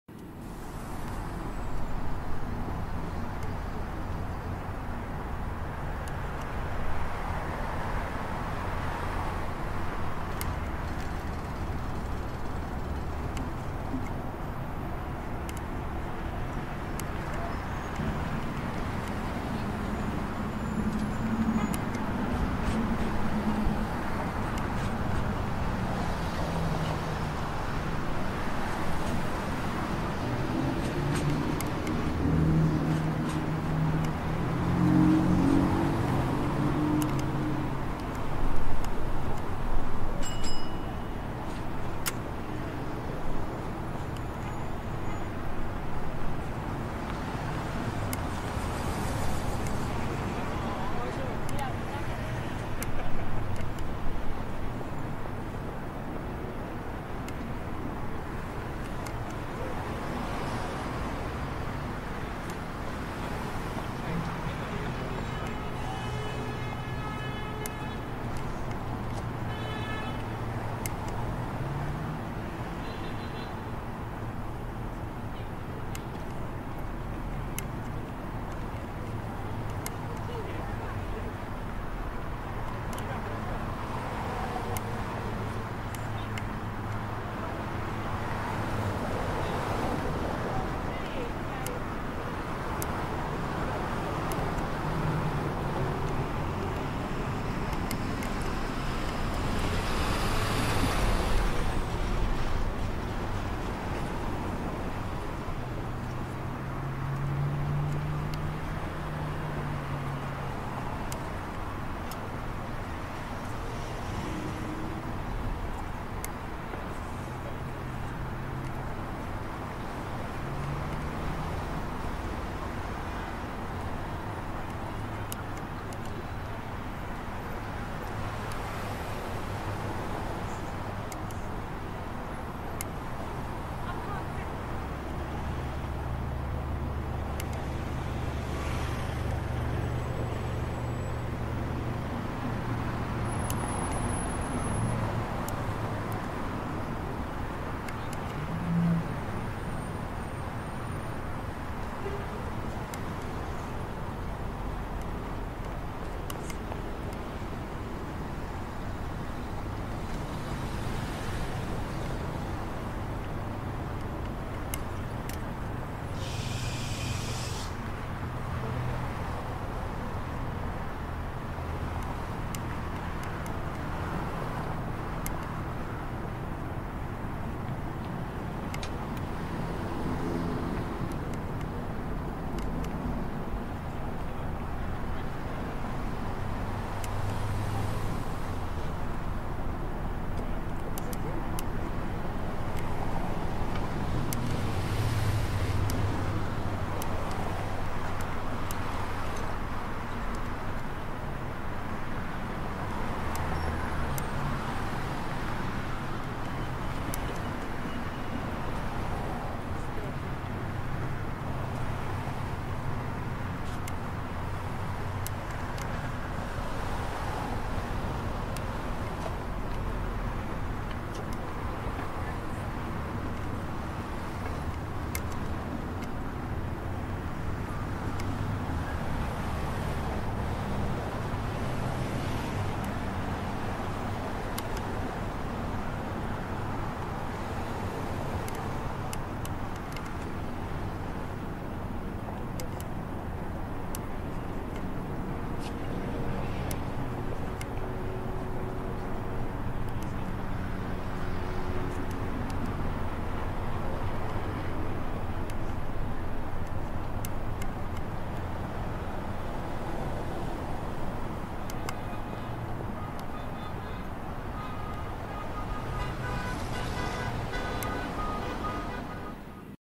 Blake Griffin Has Lunch With A Friend At The Ivy Restaurant In West Hollywood 4.23.18
Blake Griffin Has Lunch With A Friend At The Ivy Restaurant In West Hollywood 4.23.18 - TheHollywoodFix.com